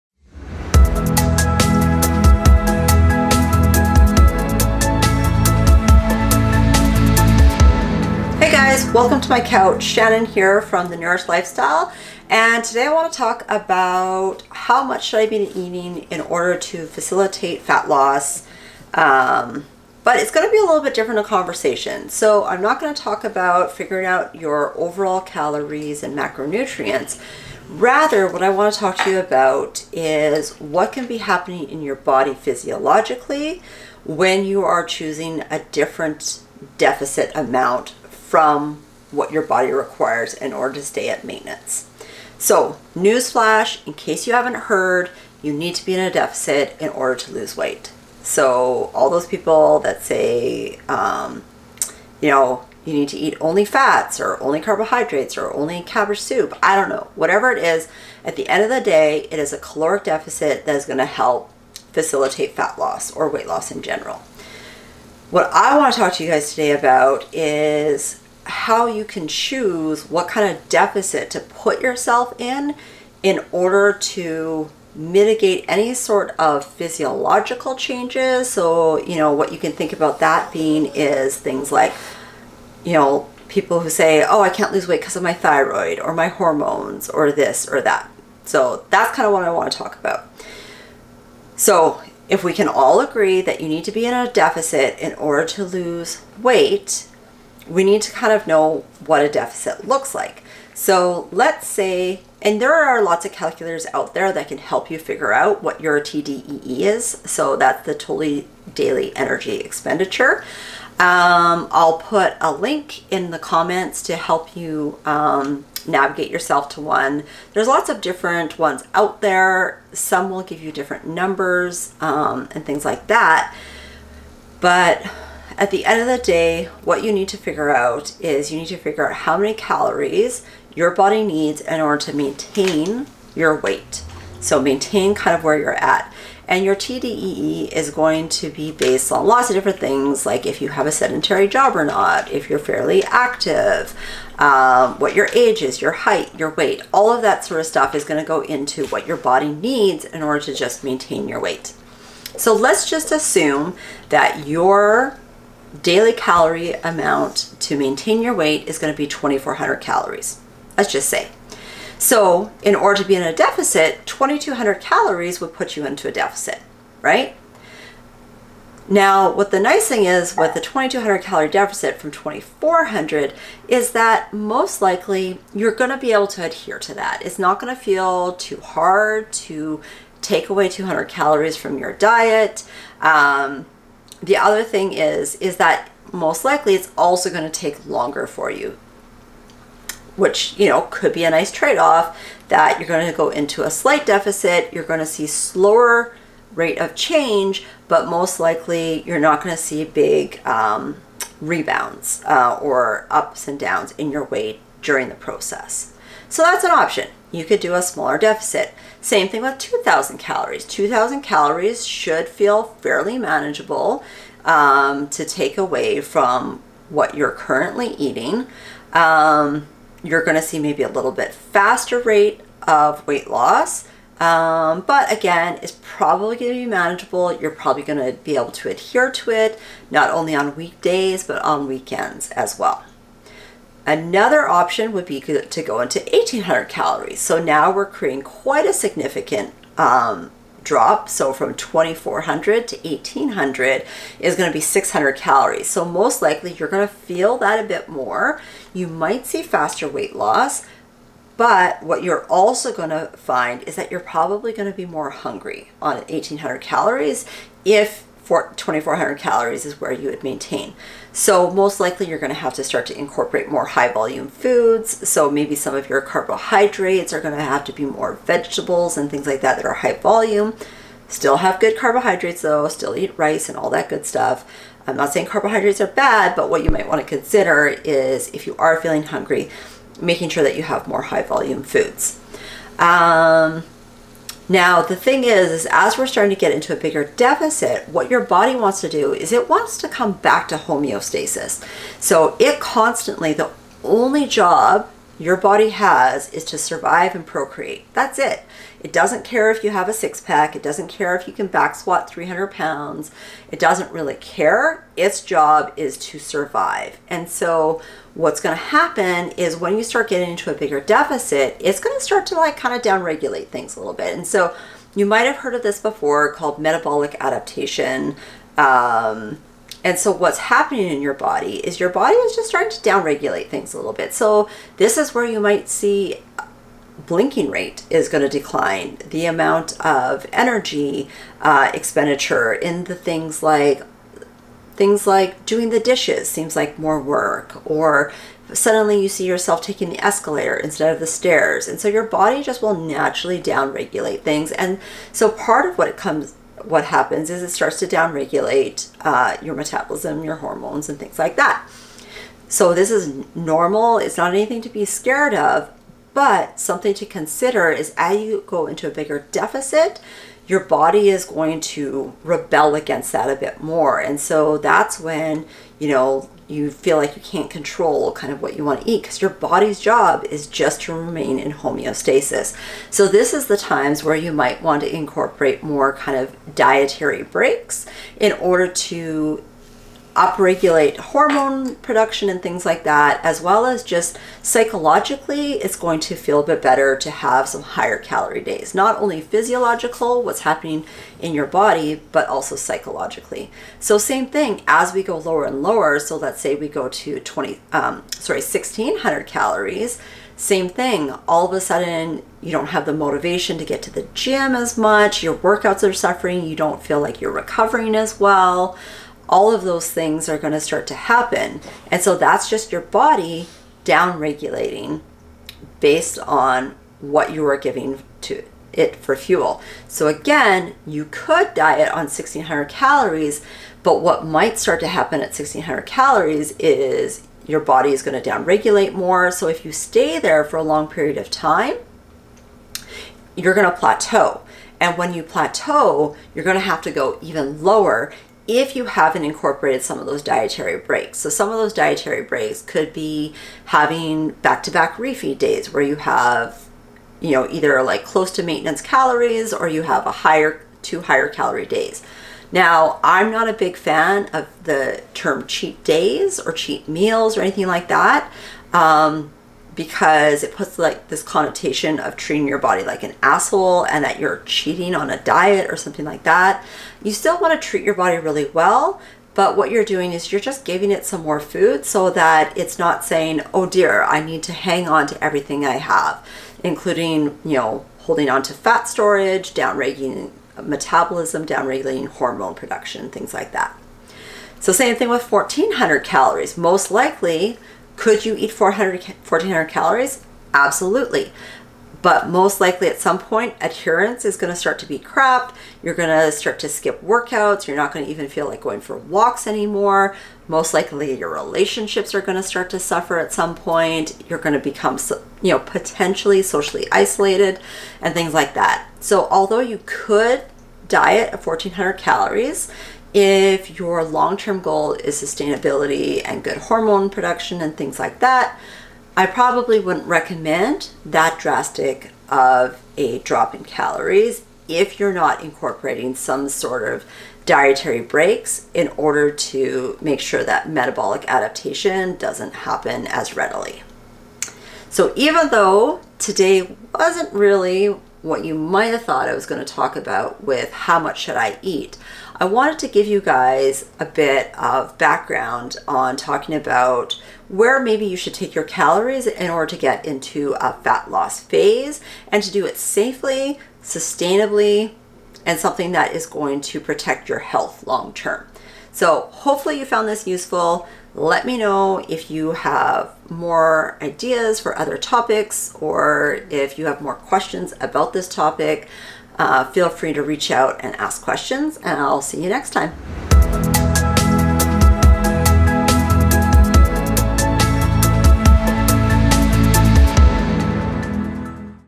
How Much Should I Eat for Weight Loss?
Welcome Back! Let's talk about how you can structure your calories for weight loss. Calories in, calories out is pretty oversimplified when it comes to how complex the human body is. Learn more about how you can structure your calories to avoid metabolic adaptation.

Here is a great calculator you can use to determine your TDEE or calorie need